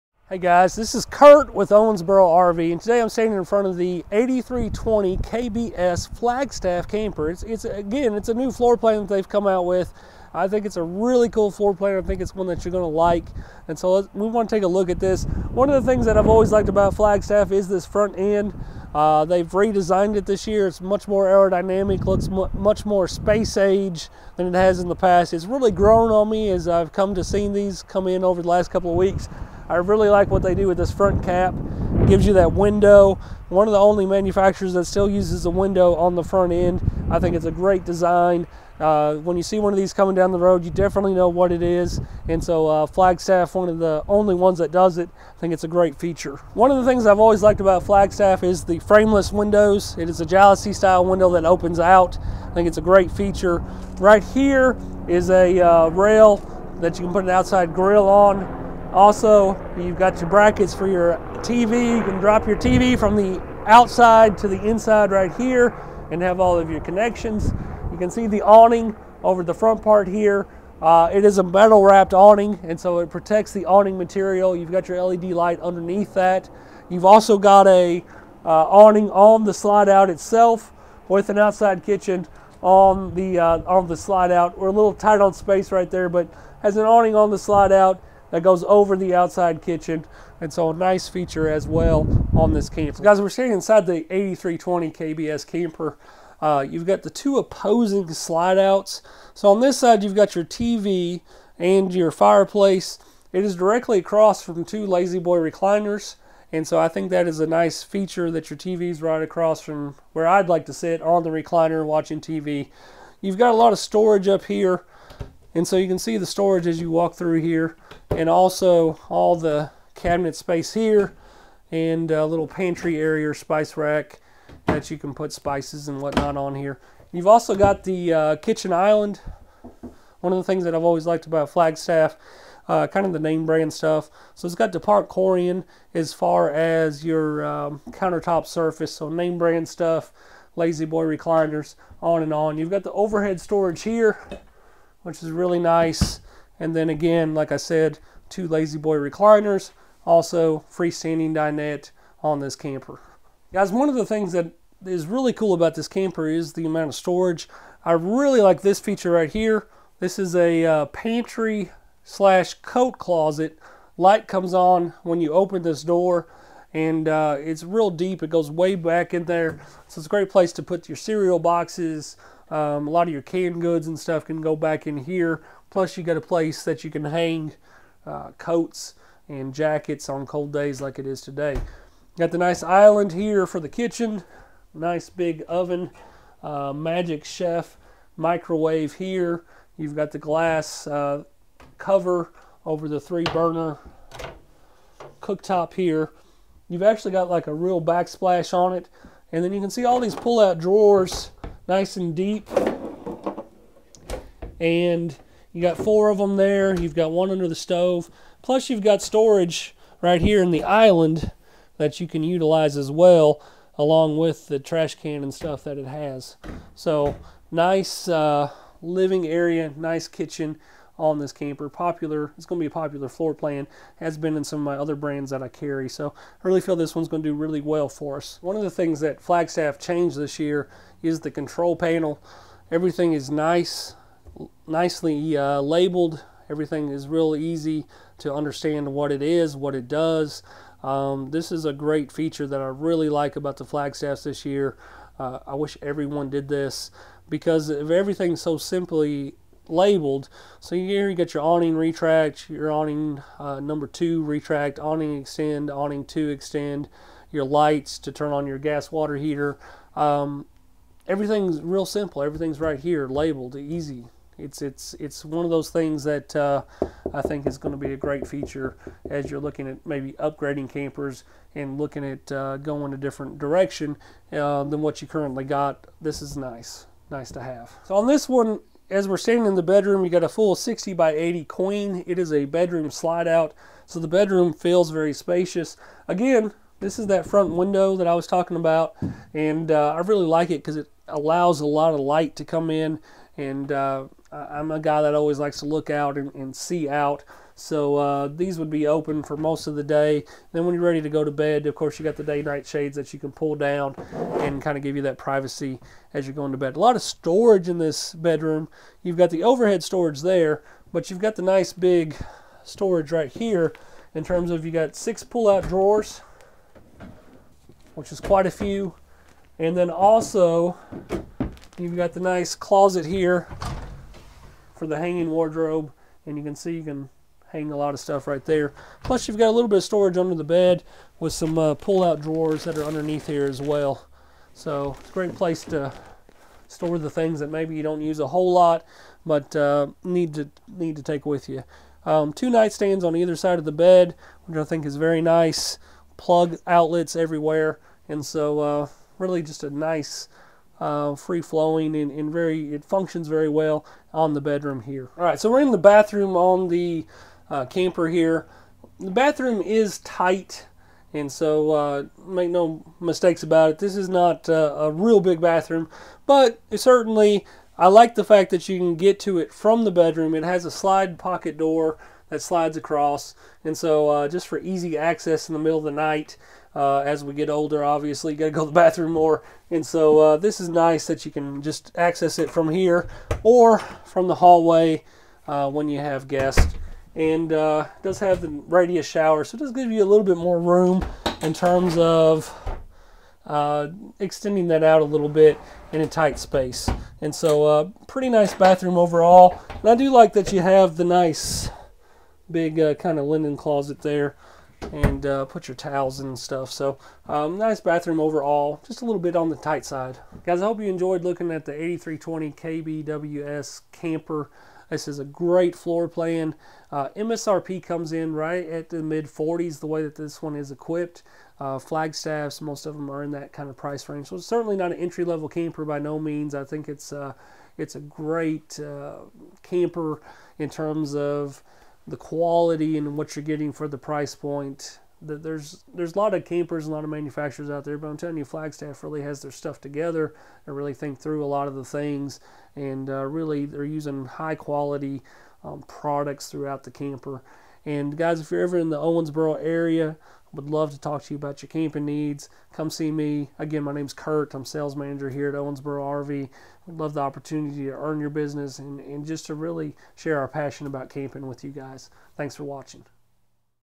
2017 Flagstaff 8320KBS Camper Review
Great floor plan from Flagstaff! This 2017 Flagstaff 8320KBS has a lot of nice features.